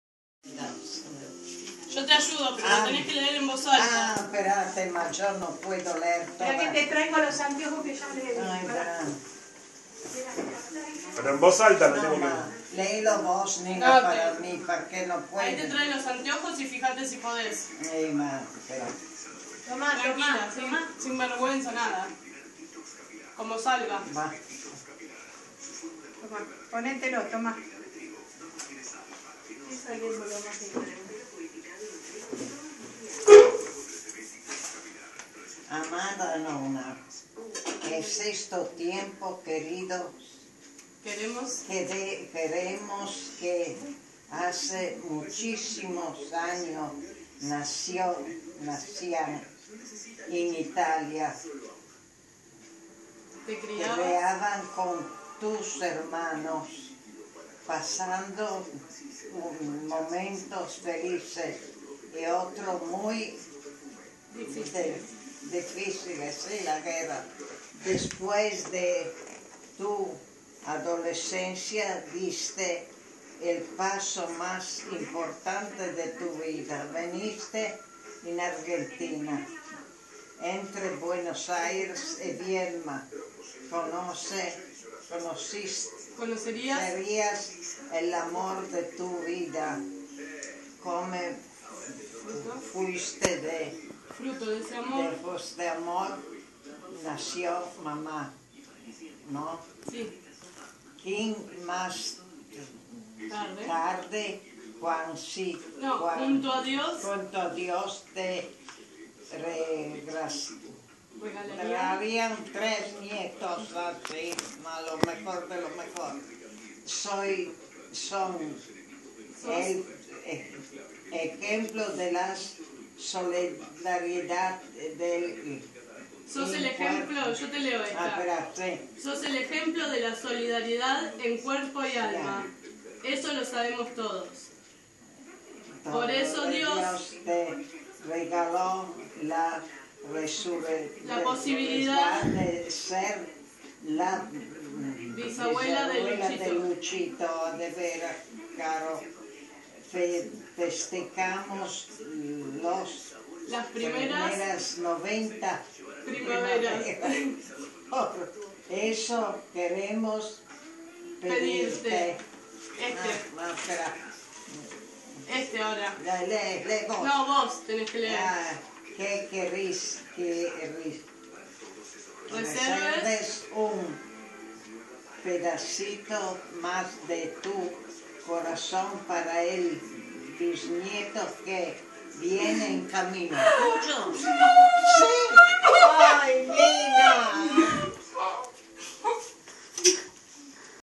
Bisabuela recibe noticia de bisnieto en camino
La nona Rita de 90 años es la que recibe la noticia familiar de la pronta llegada de su segundo bisnieto. 2017